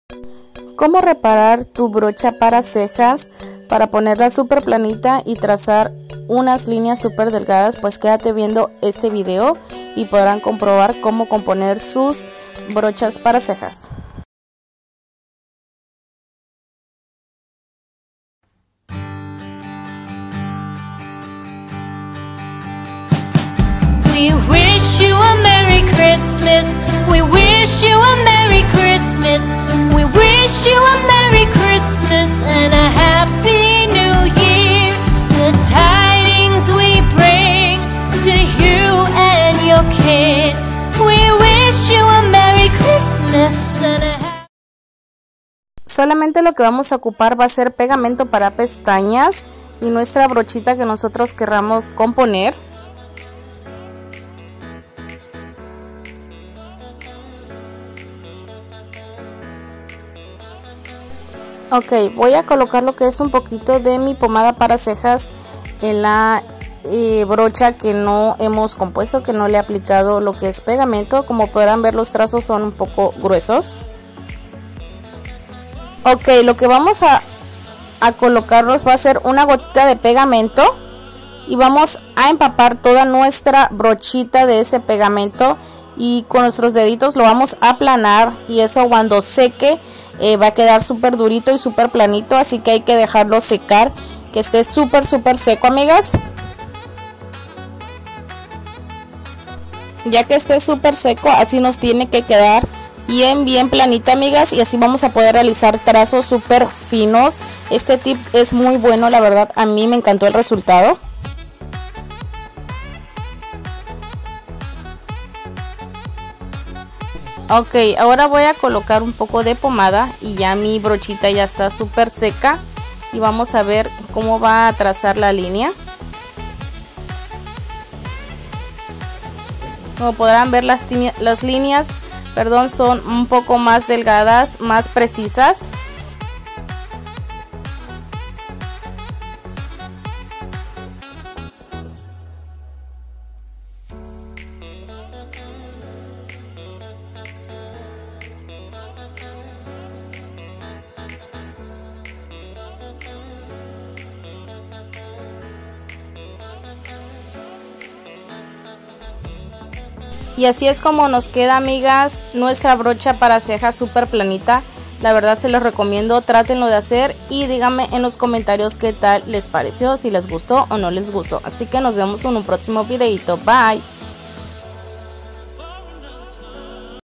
COMO REPARAR TUS BROCHAS PARA CEJAS, CON PEGAMENTO DE PESTAÑAS 😱
HOLA BUEN DIA MIS AMORES HOY LES TRAIGO UN VIDEO DE COMO COMPONER SUS BROCHAS DE CEJAS CON SOLO PEGAMENTO PARA PESTAÑAS. SII TE QUEDARÁS SORPRENDIDA DE LO QUE PUEDE PASAR, ASI QUE QUEDATE VIENDO ASTA EL FINAL 😍